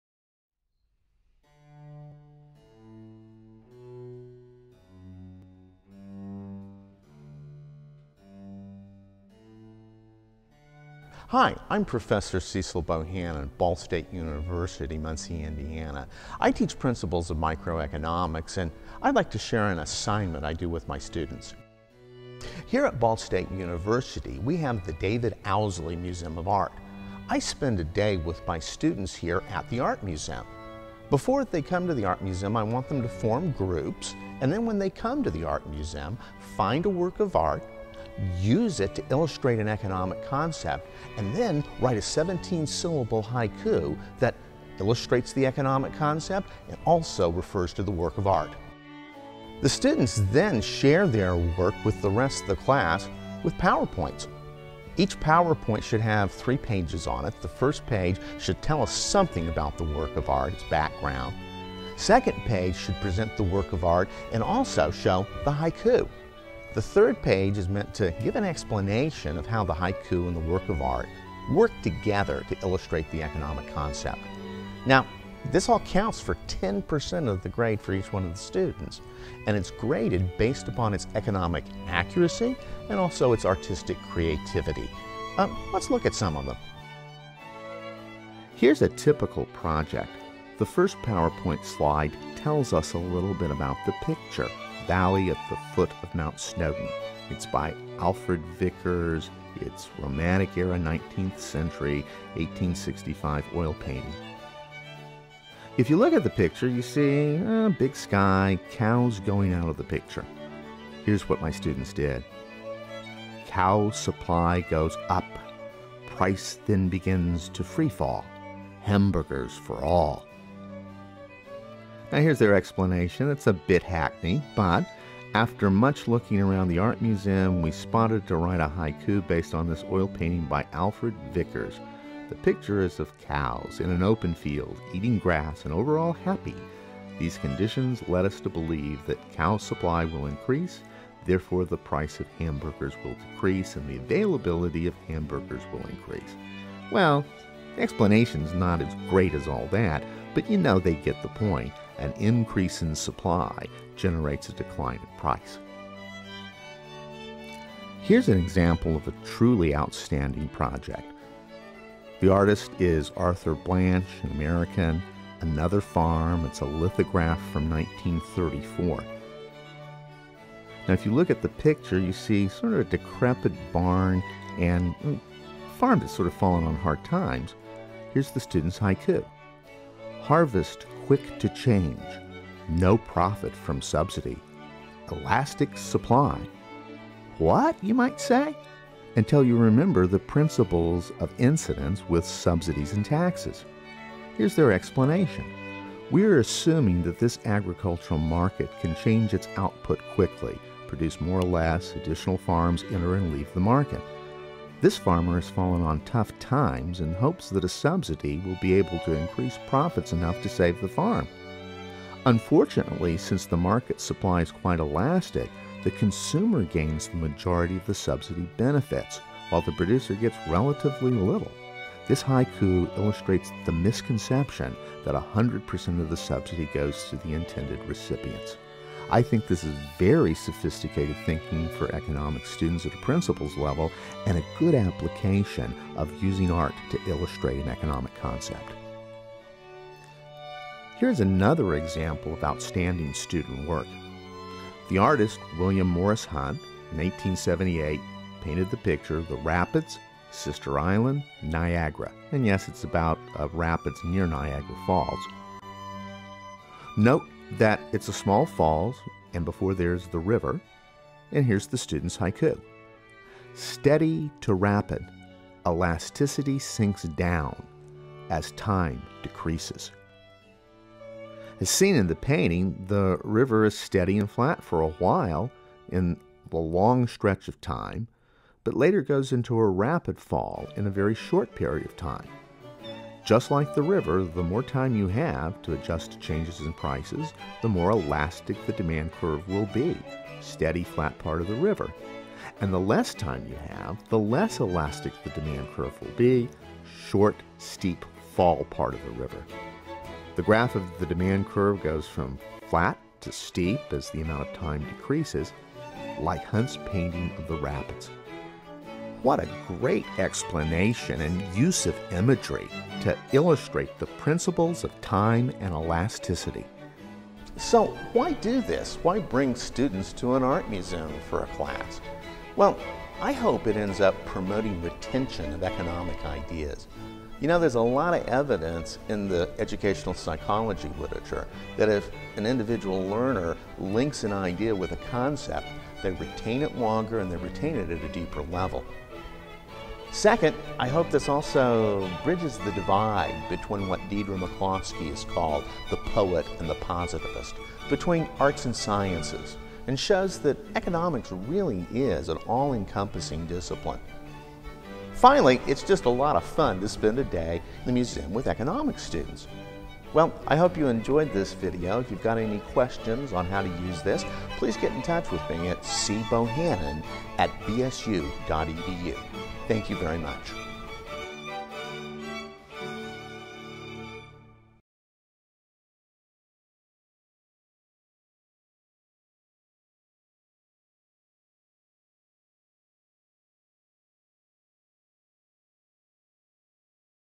Art, Haiku, and Economics
Dr. Cecil Bohanon explains economics by bringing his students to the art museum.